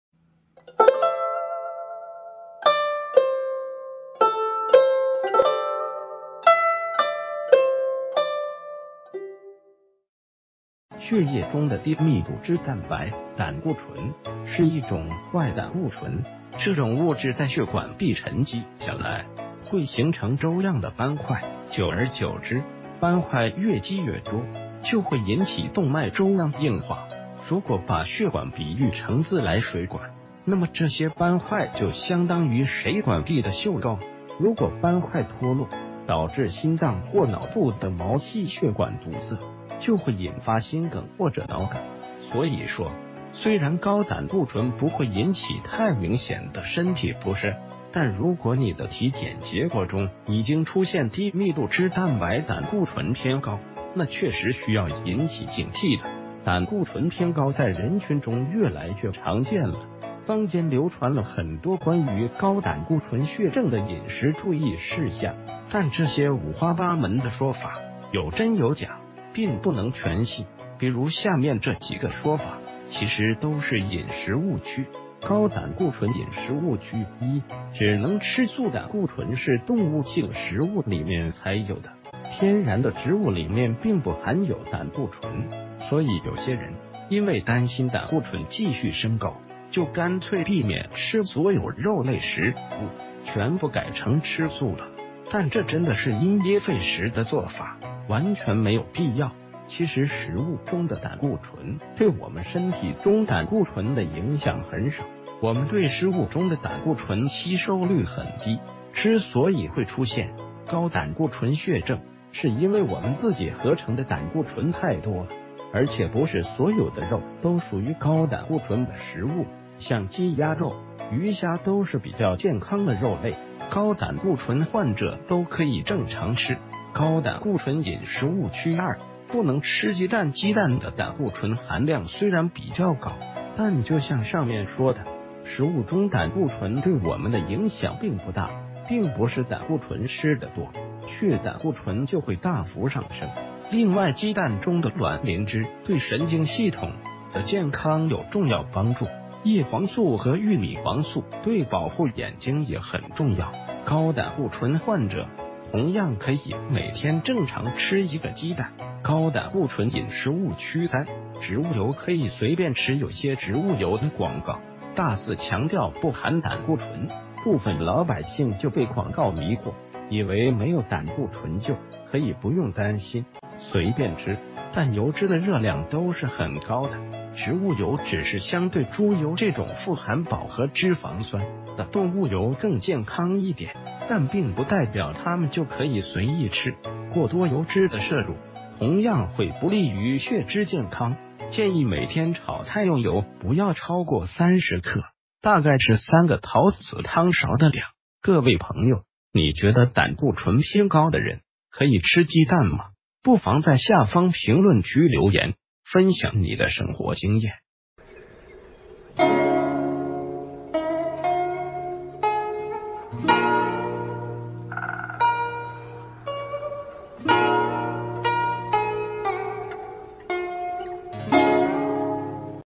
膽固醇偏高的人，不能吃肉嗎？營養師：降膽固醇時不要犯3個錯誤
血液中的低密度脂蛋白...